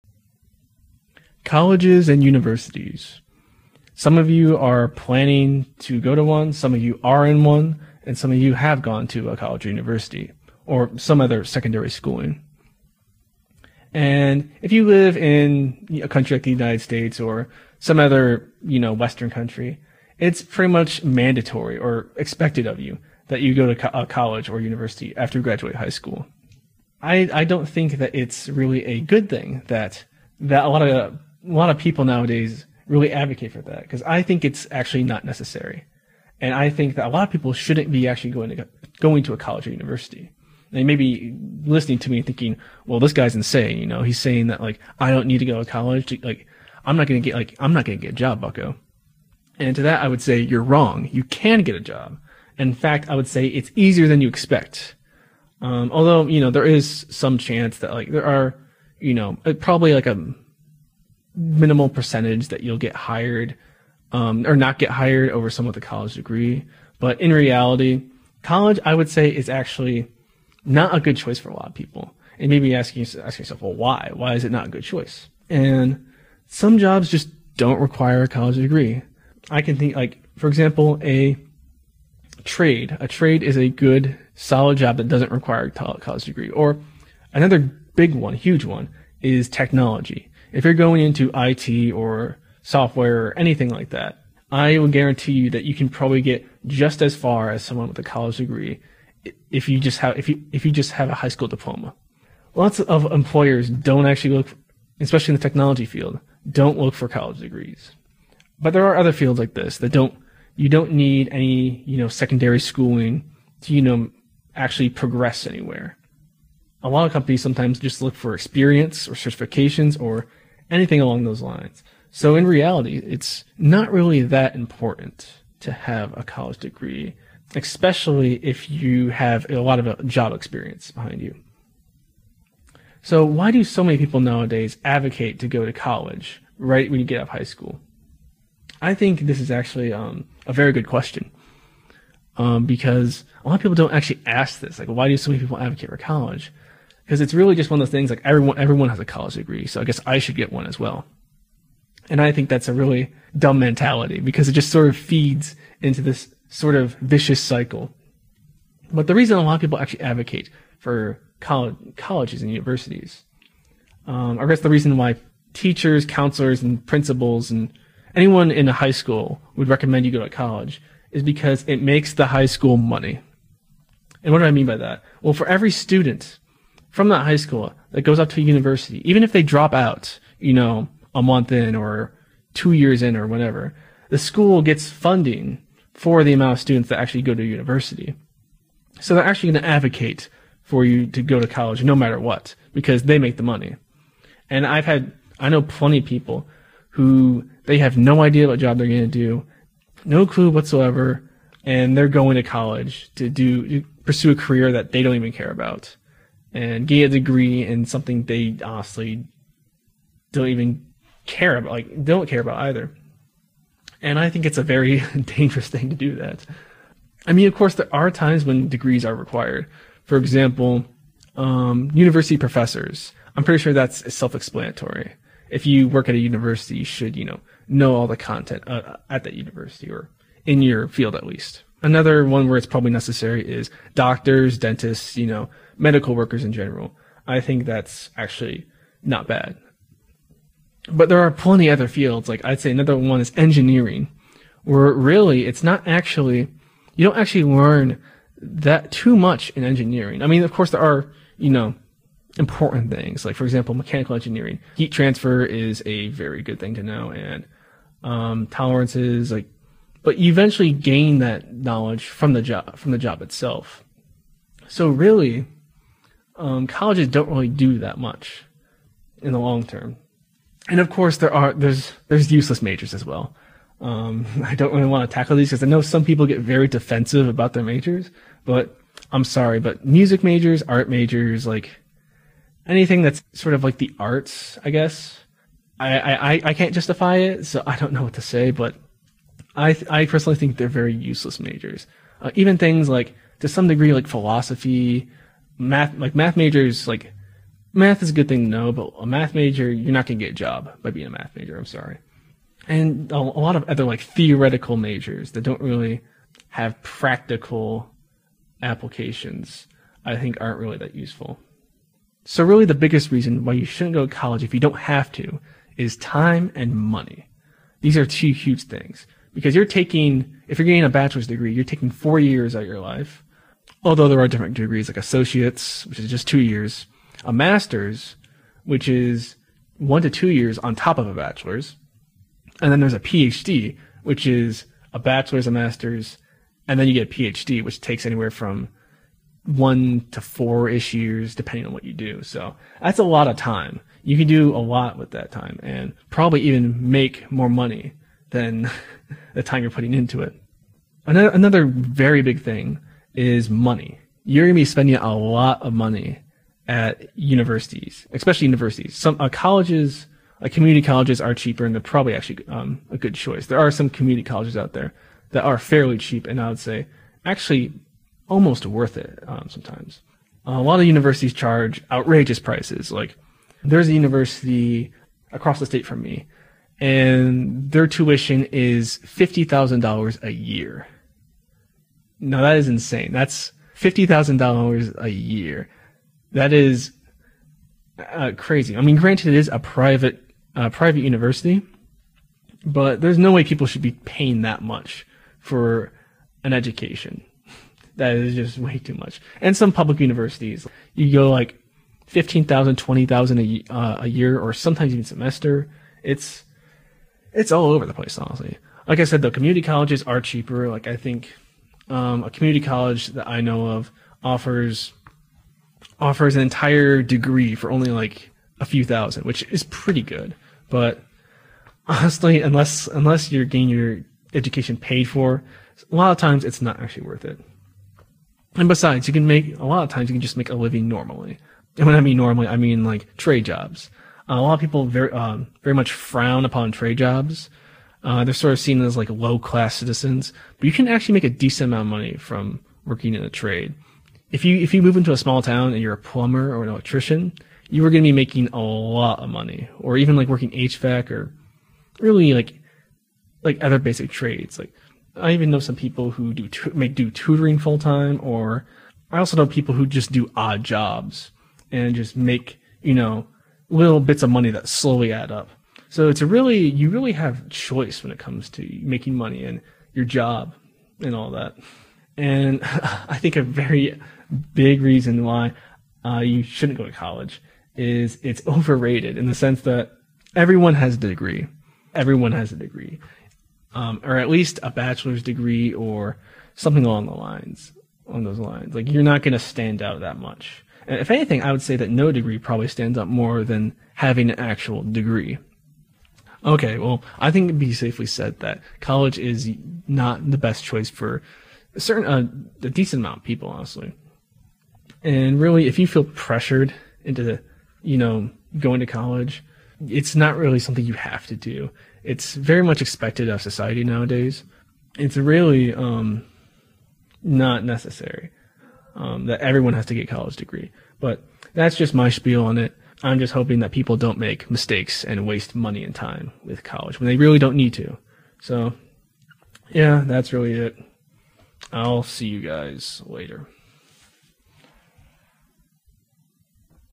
Should you go to College?
Should you go to college? Is it even a good thing? I will be discussing the reasons why some people should not be going to a college or university